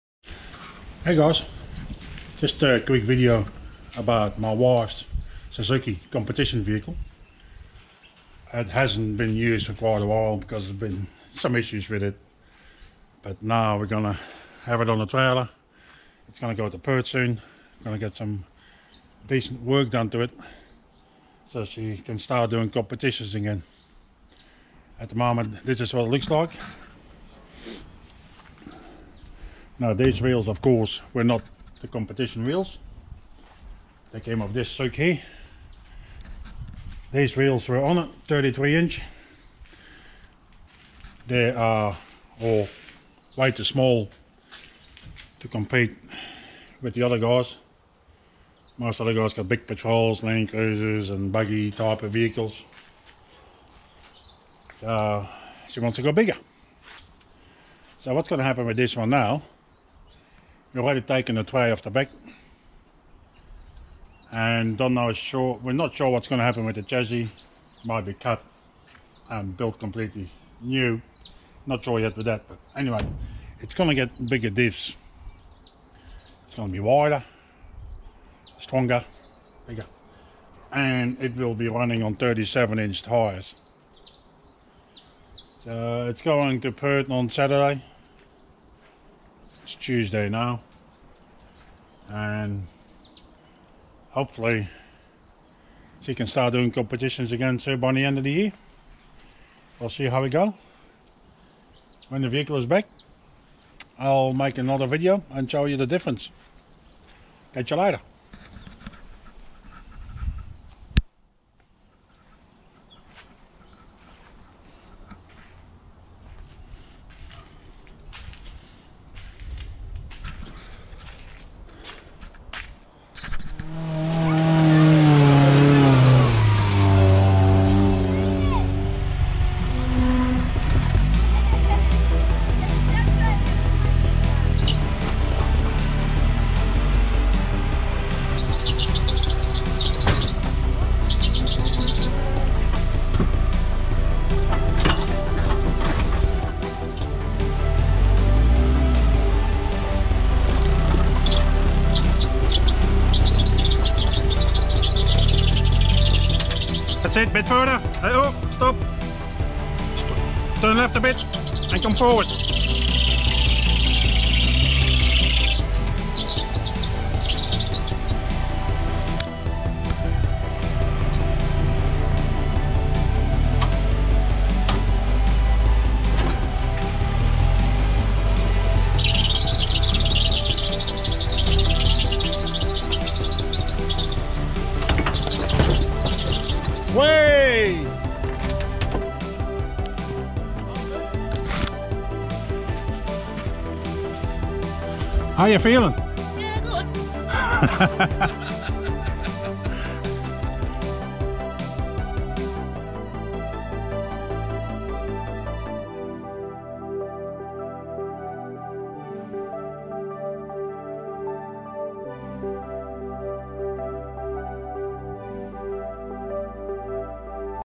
The Zook
My wife's Suzuki, competition vehicle, will be going to Perth to get major work done to it.